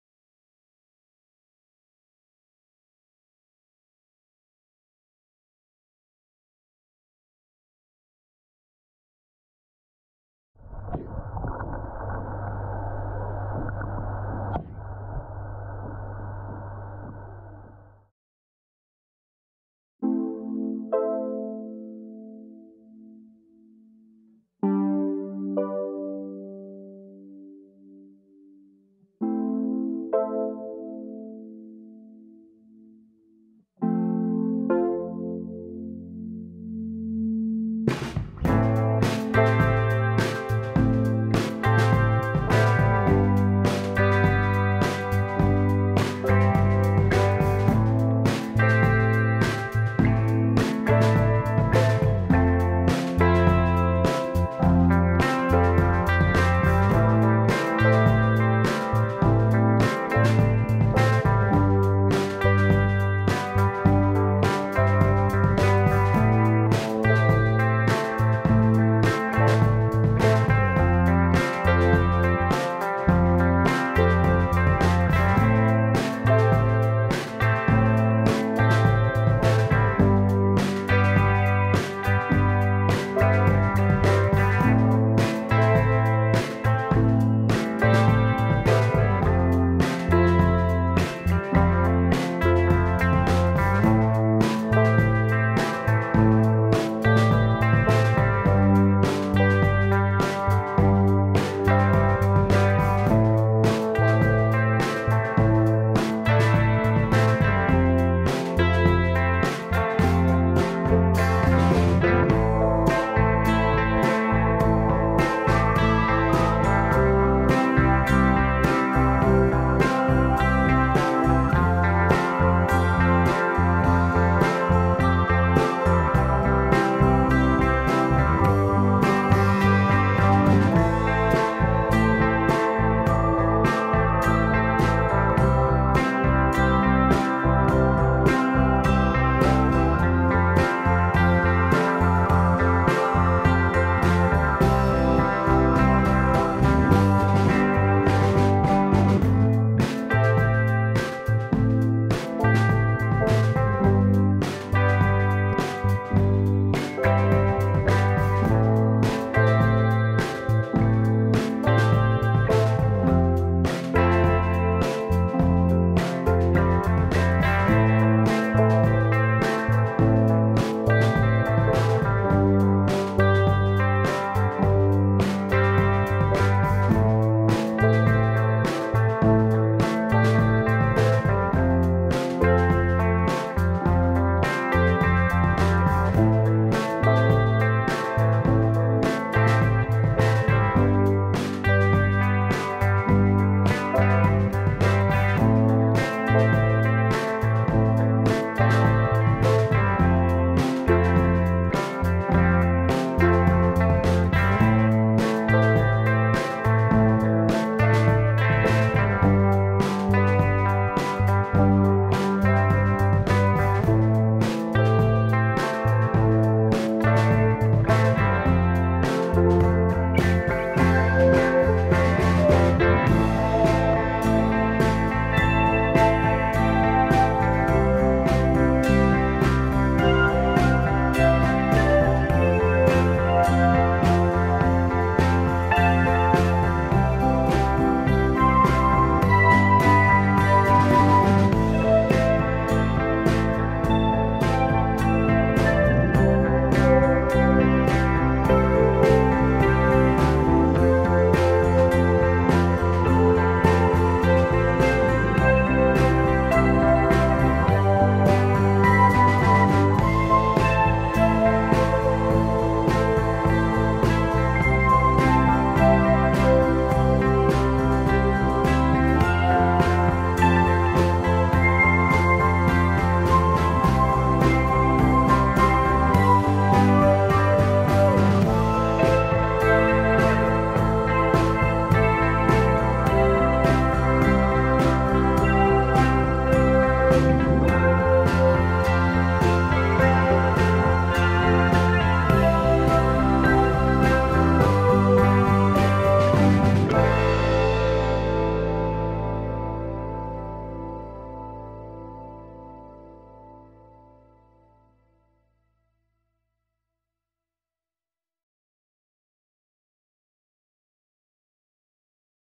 Our Koi - 1st June 2023 - 4K
This short video with underwater footage of our Koi. This was recorded late evening on 1st June 2023. The Koi were hungry as always but otherwise calm.